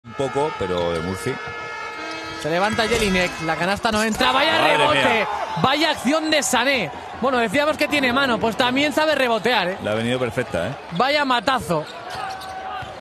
Potente REBOTE y MATE de Landing SANÈ | Liga Endesa
El pívot francés enmienda el error de David Jelínek con un potente mate en las alturas. ¡No te pierdas el putback de Sanè!